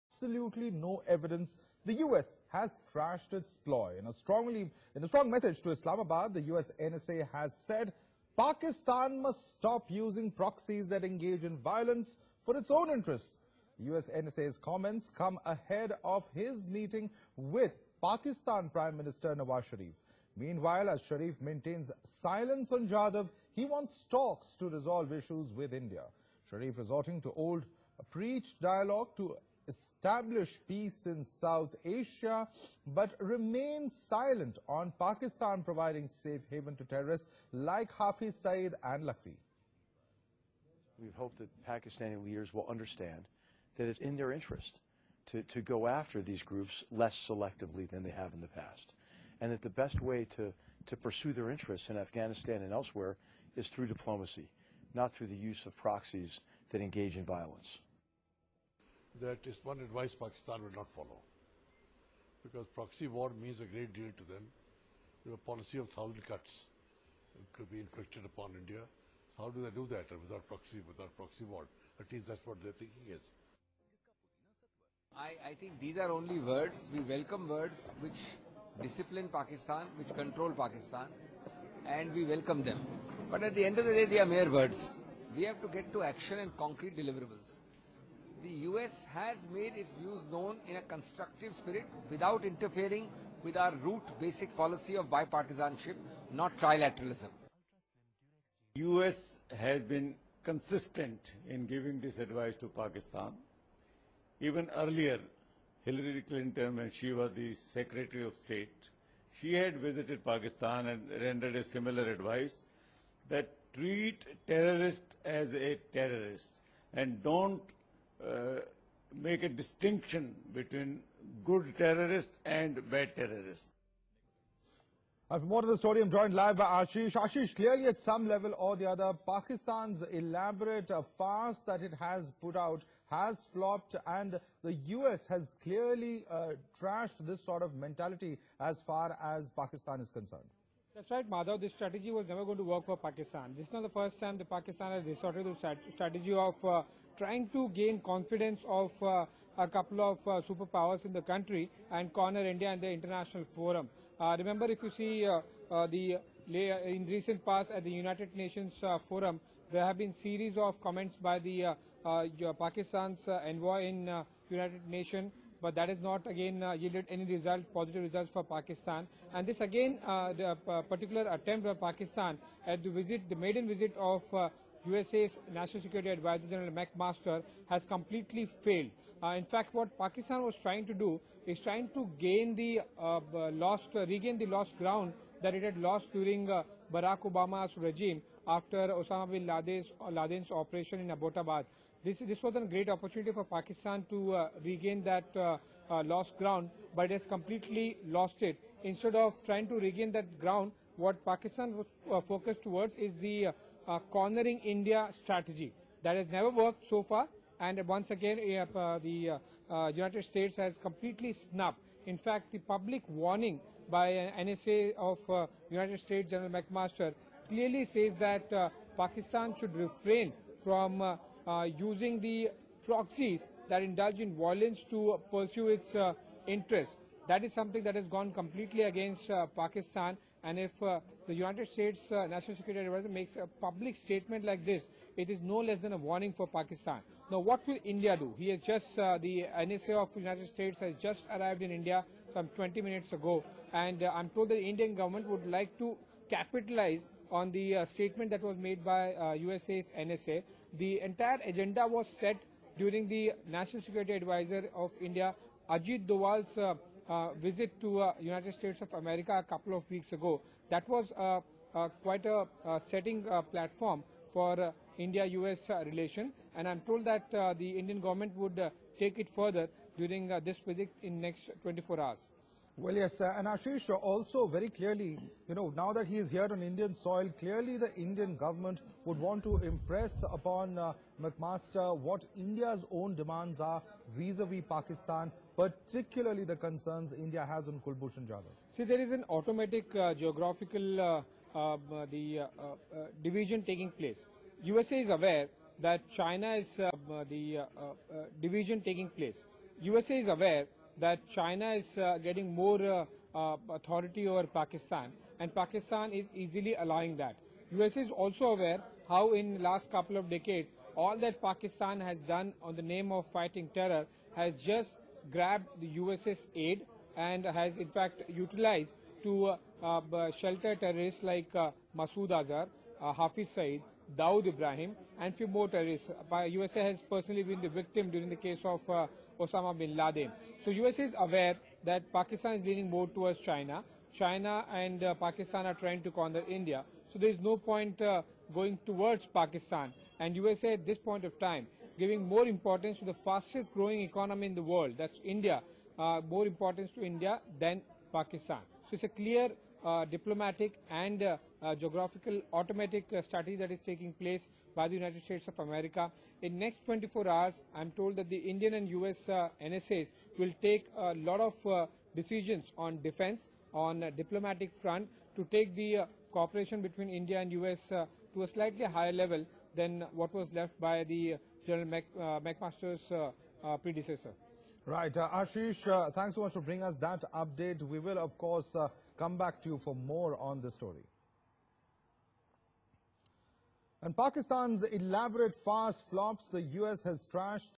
Pak talks Peace but has no evidence against Jadhav
Pak PM Nawaz Sharif meeting US NSA McMaster , expressing that Pak wants a peaceful relationship with India.